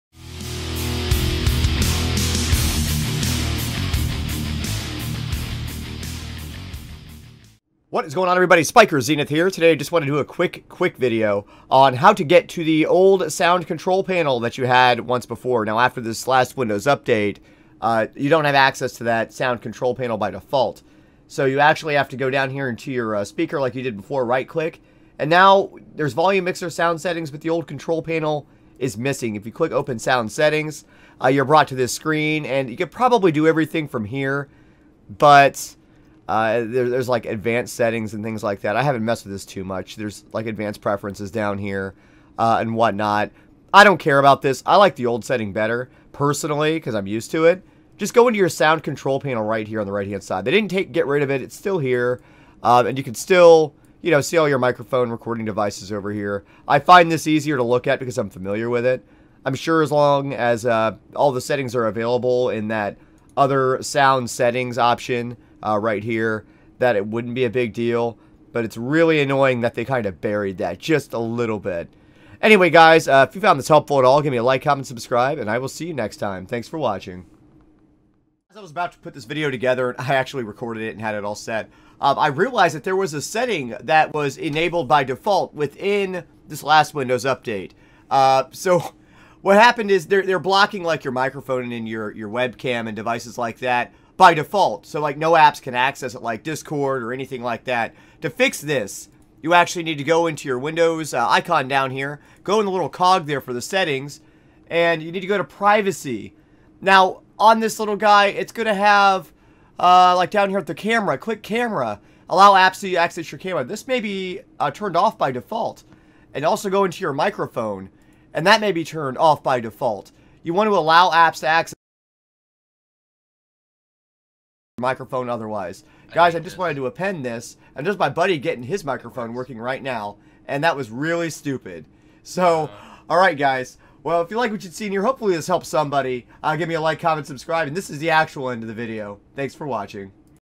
Windows Update Mic Fix 2018
There was a recent update that affected quite a few users and this is a short video on how to get your microphone and webcam to work again with discord, skype etc.

-=SpykerZ=-Xenith